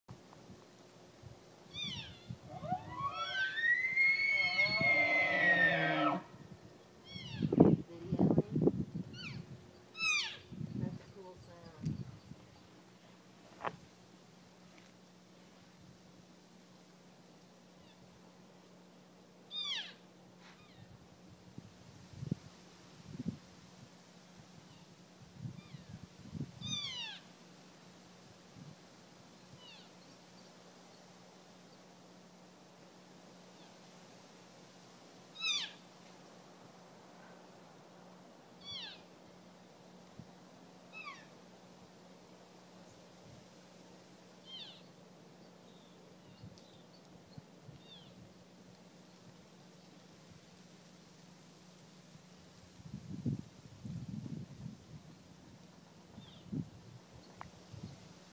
Elk in our backyard
Every fall the elk heard gathers together and is right behind our house almost daily. This was taken October 5th, 2011